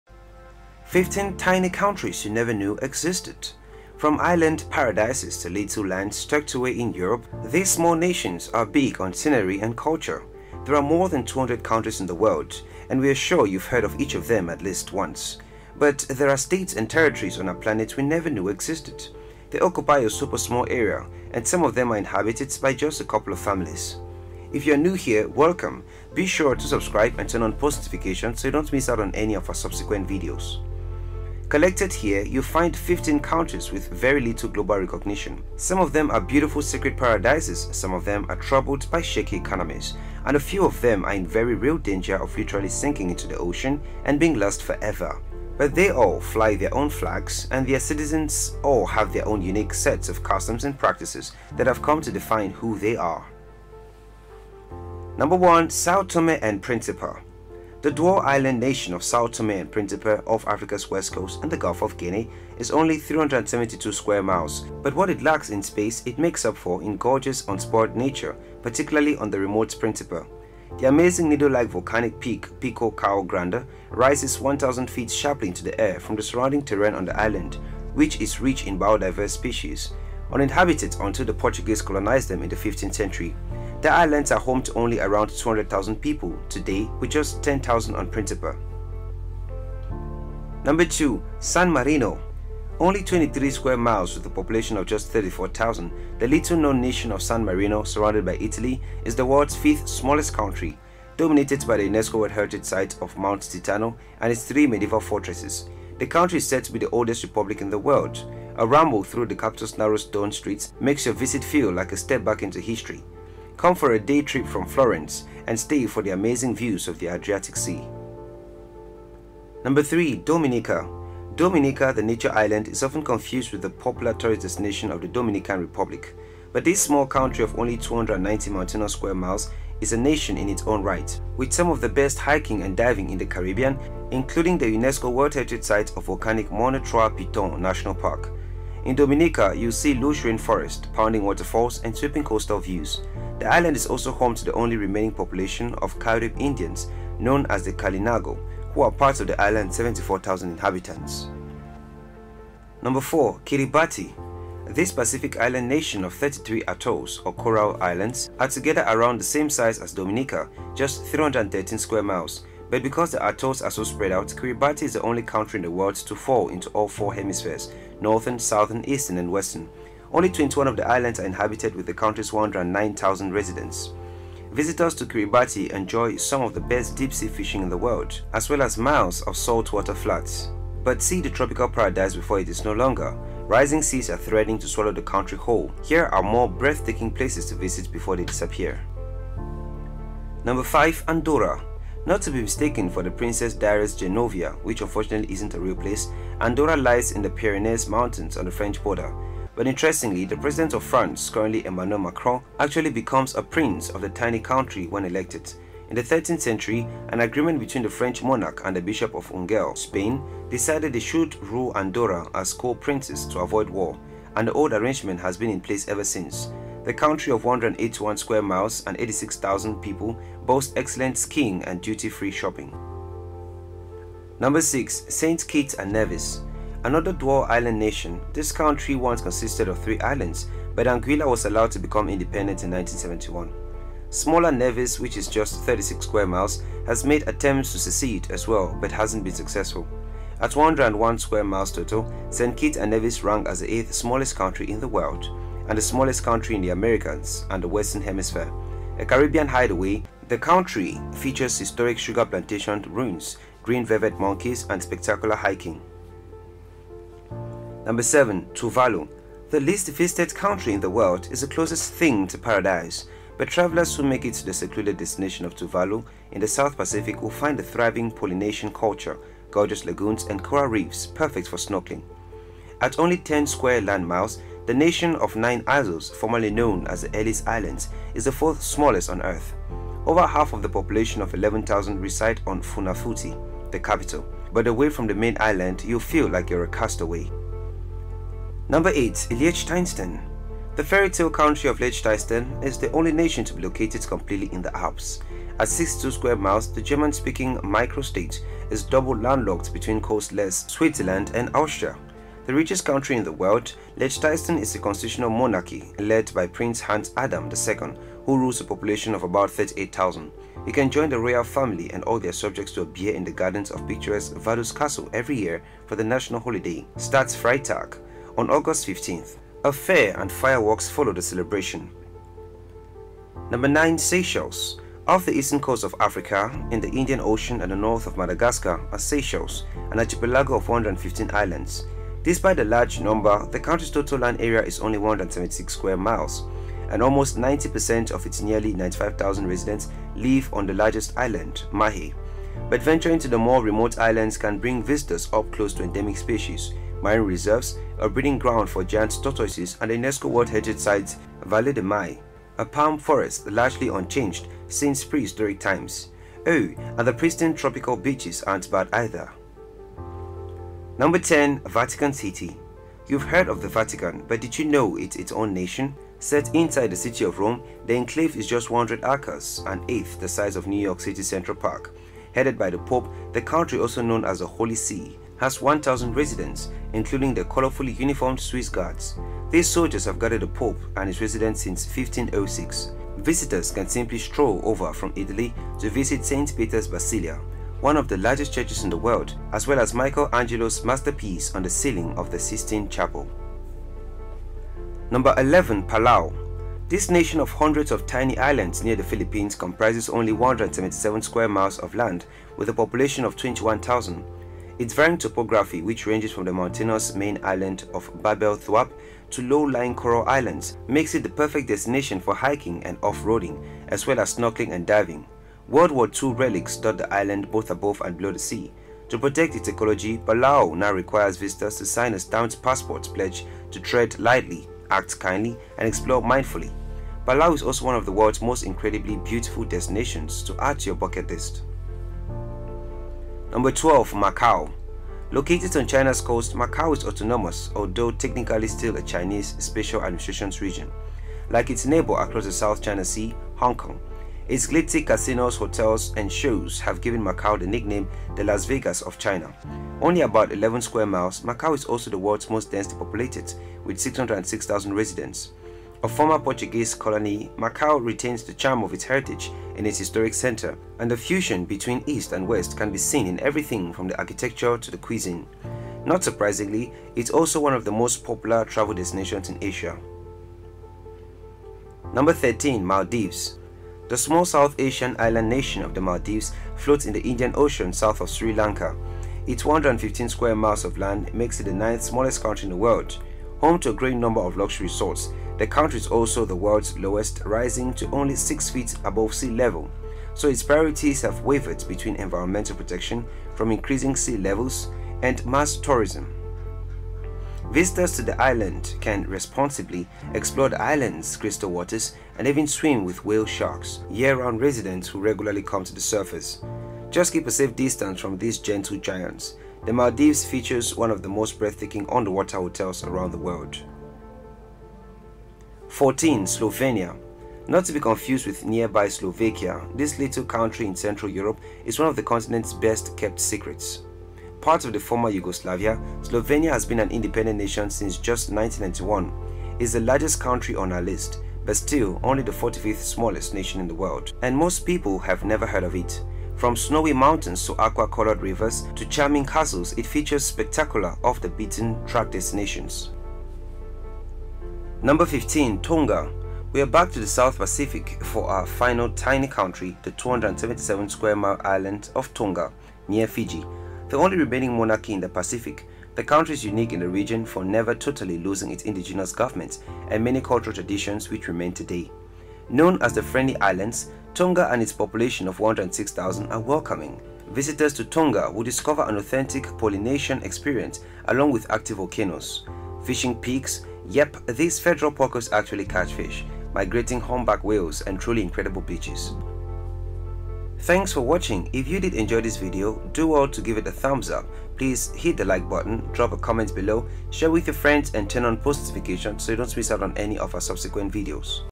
15 Tiny Countries You Never Knew Existed
There are so many countries in the world and just by the mention of them we know its a country because we are all familiar with them. But there are some countries that you might not have known that existed. In this video, we are going to be seeing 15 tiny countries you never knew exited.